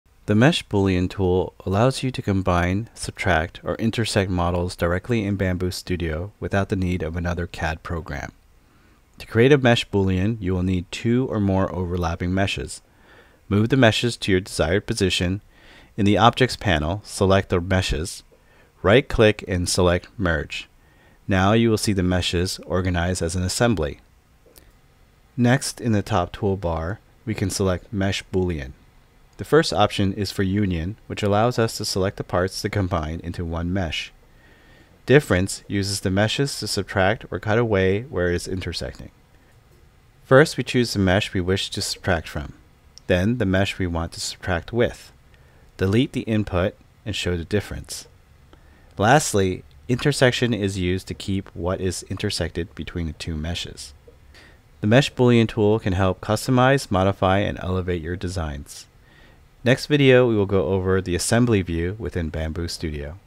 Bambu Studio Guide: Mesh Boolean Tool
In this tutorial, learn how to use the Mesh Boolean Tool in Bambu Studio to combine, subtract, or intersect 3D models directly within the software—no additional CAD programs needed!
With this powerful feature you can customize and elevate your designs with ease. Whether you’re merging parts, cutting away material, or isolating intersecting areas, this tool simplifies complex modifications.